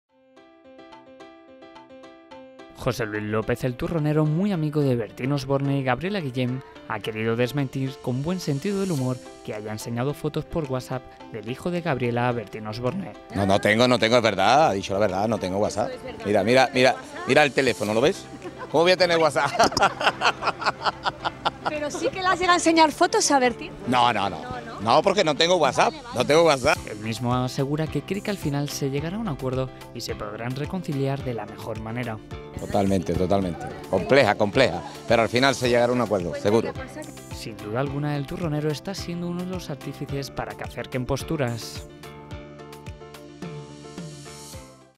'El Turronero' se moja y cree que Bertín y Gabriela llegarán a un acuerdo
José Luis López, 'El Turronero', muy amigo de Bertín Osborne y Gabriela guillen ha querido desmentir con buen sentido del humor que haya enseñado fotos por WhatsApp del hijo de Gabriela a Bertín Osborne.